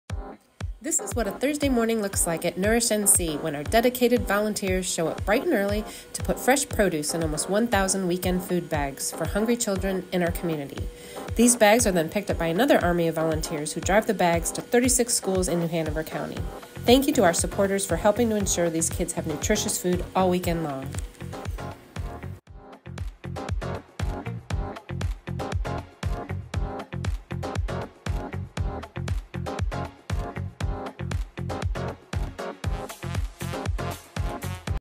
Thursday morning at NourishNC!
A typical Thursday morning at NourishNC! Our dedicated volunteers are adding produce to 1,000 weekend food bags for hungry children in our community. These bags will be delivered to 36 schools right here in our community!